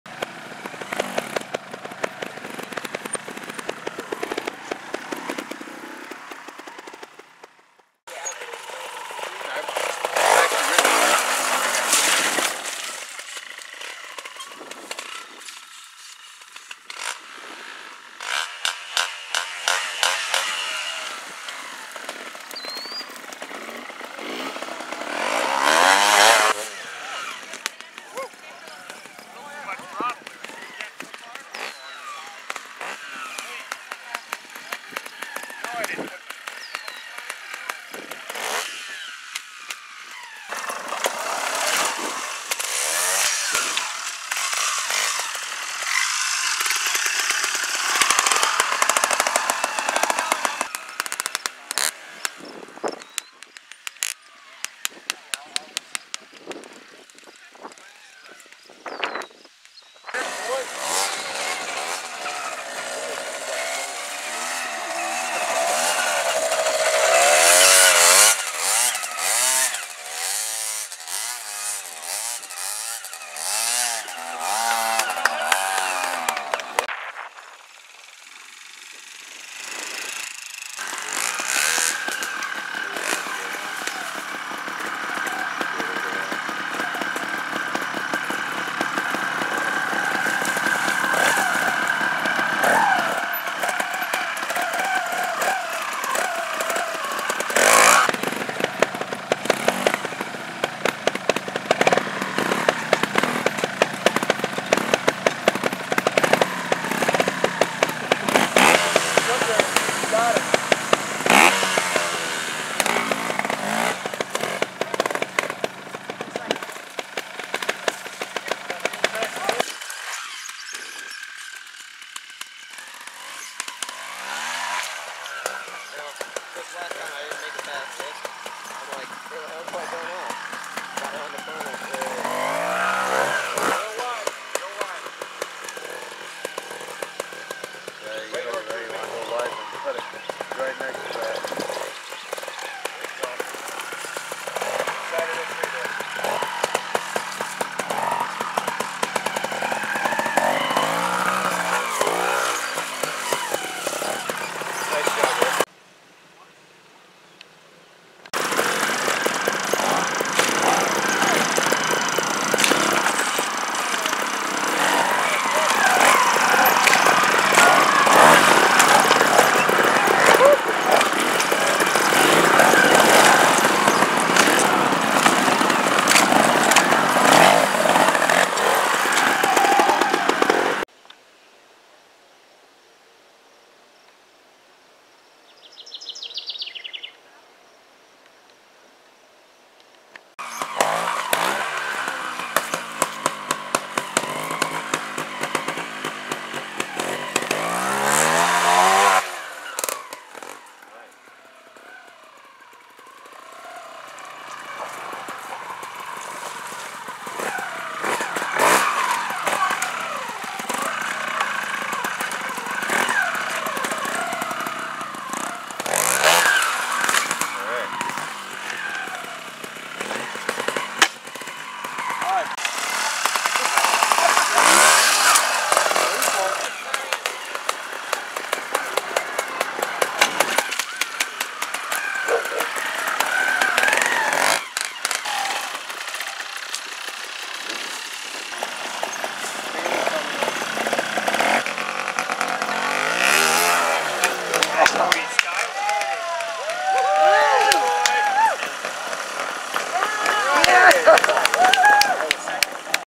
How to trials ride in Little Sturgeon WOTA
A beautiful day in Little Sturgeon Bay, Wi at the Gustafson residence as they were hosts to the 1st annual WOTA (Wisconsin Observed Trials Association) event on 6/6/15. The day was sunny, 65 degrees, and very thoroughly enjoyed by all who attended. I pieced together this video which shows many different drivers on four different sections of the trials riding course, and also one play area. You will enjoy seeing some triumphs and tribulations of the riders on this course. This is definitely a "How to" video as all who were involved in setting up and operating this event did a wonderful job.